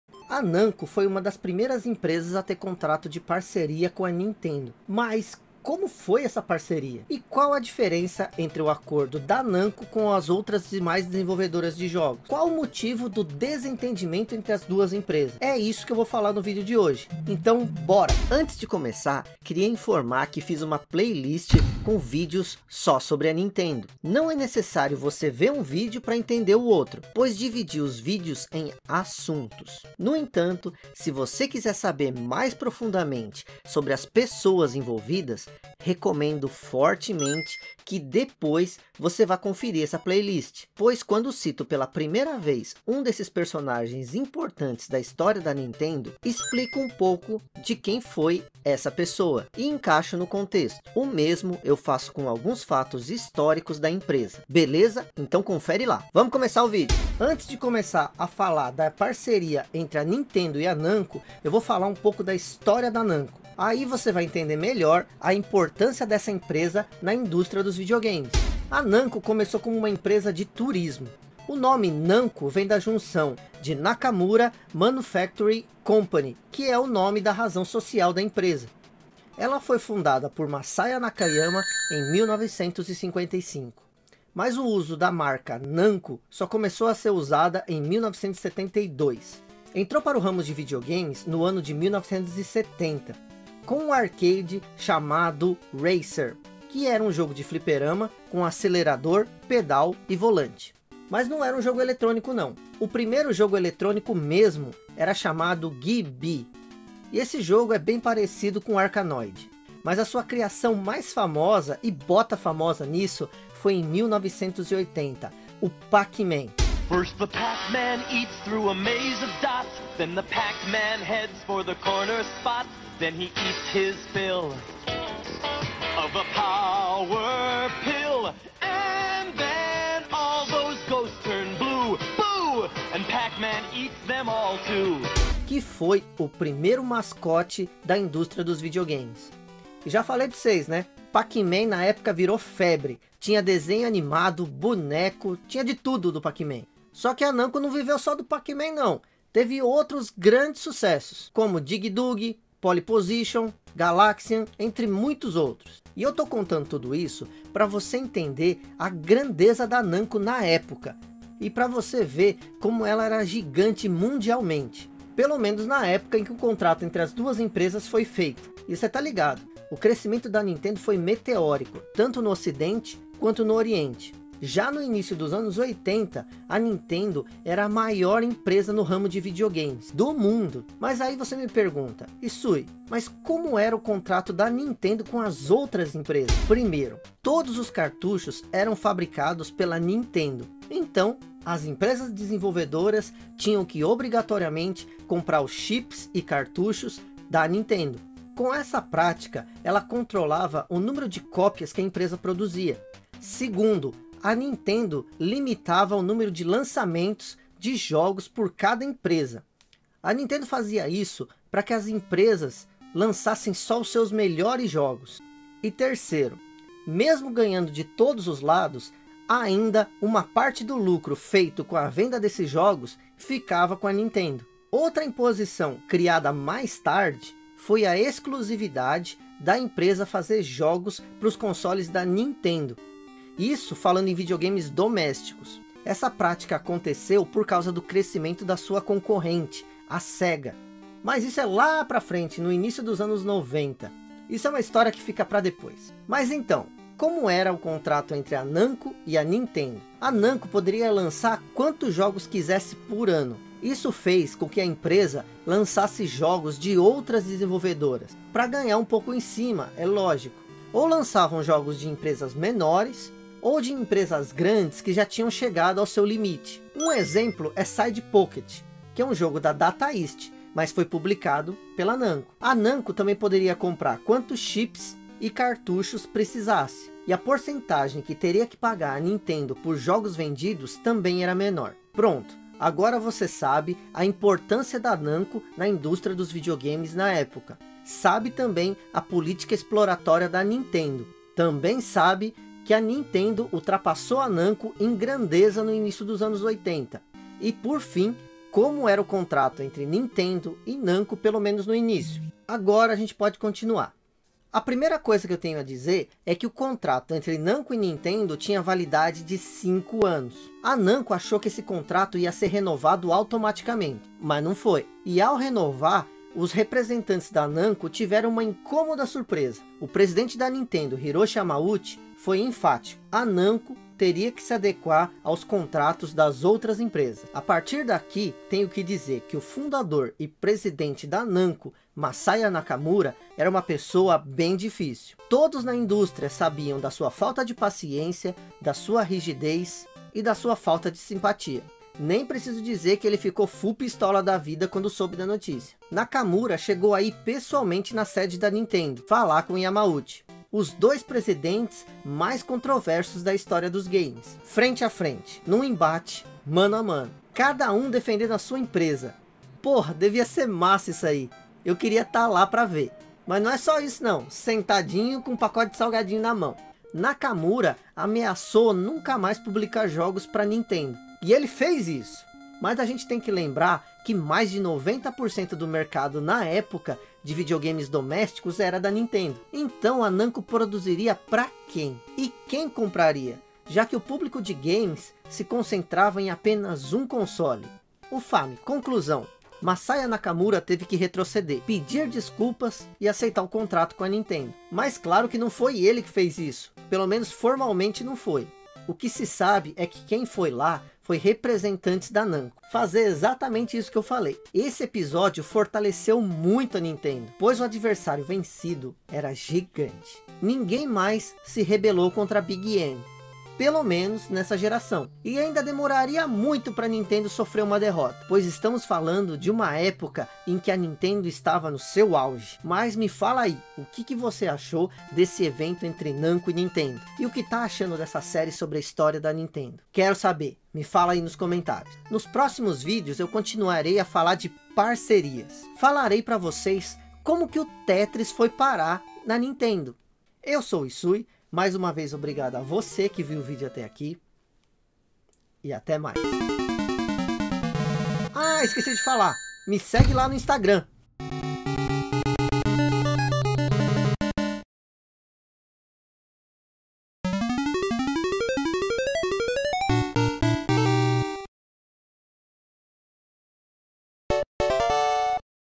Como foi a parceria entre NAMCO e NINTENDO ?
A Namco foi uma das primeiras empresas a ter um contrato de parceria com a Nintendo, mas como foi essa parceria ?
E qual a diferença do acordo entre a Namco e as demais desenvolvedoras de jogos da época ?
Qual o motivo do desentendimento entre as empresas ?
È isso que falarei no vídeo de hj
Bora